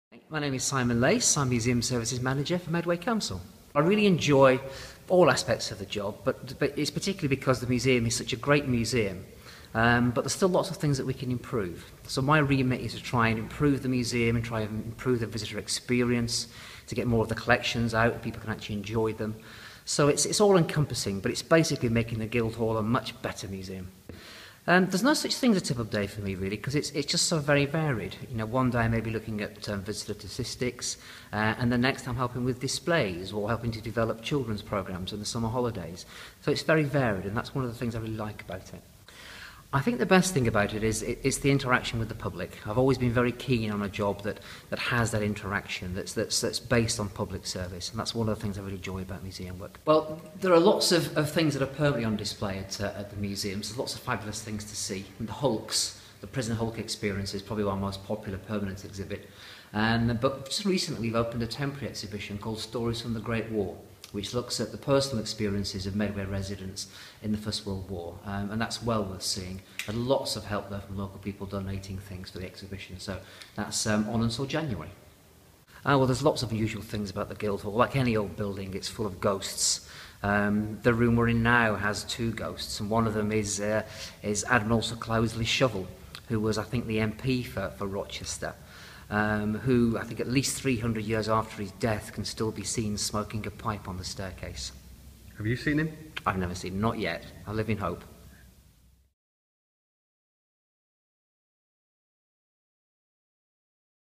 Meet the team - the man behind the museum
Simon Lace is Medway Council's Museum Services Manager